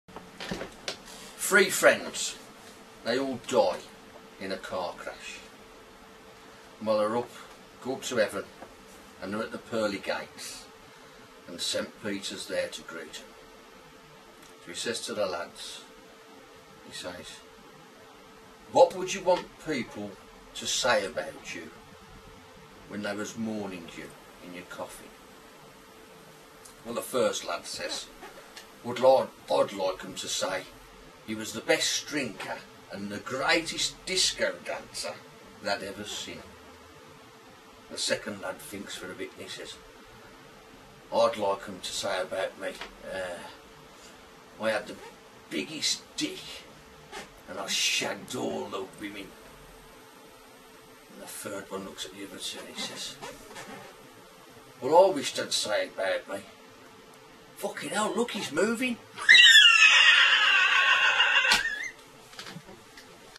What would you want people to say about you when your dead?
When your lying in your coffin and people are mourning you what would you want to hear them say about you?